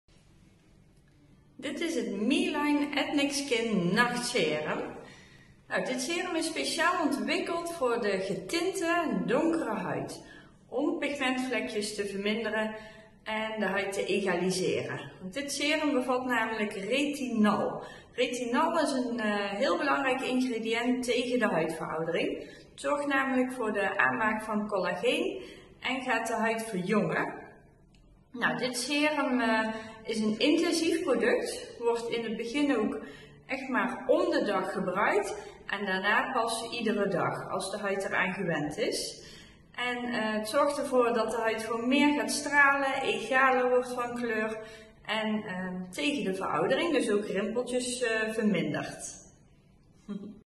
Meline Ethnic Night, Intensief nacht serum voor donkere huidtypes, breekt te veel pigment af.
Intensief nacht serum voor donkere huidtypes, breekt te veel pigment af.
Niet geschikt tijdens zwangerschap of borstvoeding periode.

MeLine Ethnic Skin Night is speciaal ontwikkeld om ’s nachts te werken aan een egale teint en het verminderen van hyperpigmentatie bij een donkerdere huid.
De formule bevat krachtige ingrediënten zoals retinol en niacinamide die de huidtextuur verbeteren en de melanineproductie reguleren.
Door regelmatig gebruik van MeLine Ethnic Skin Night worden donkere vlekken en verkleuringen geleidelijk verminderd, terwijl de huidteint egaler wordt.
Werkstoffen MeLine Ethnic Skin Night:

Retinol: Stimuleert de celvernieuwing, vermindert hyperpigmentatie en verbetert de huidtextuur.
Niacinamide (Vitamine B3): Vermindert donkere vlekken, egaliseert de teint en versterkt de huidbarrière.
Arbutine: Remt de productie van melanine en helpt bij het verminderen van hyperpigmentatie.
Licorice Extract (zoethoutextract): Werkt als een natuurlijke huidoplichter en vermindert de zichtbaarheid van donkere vlekken.
Vitamine C: Krachtige antioxidant die de huid verheldert, beschermt tegen vrije radicalen en de collageenproductie stimuleert.

Wil je anti-aging producten bestellen, zodat je je huid thuis natuurlijk jonger maakt?

Breitnerstraat 2a
Eindhoven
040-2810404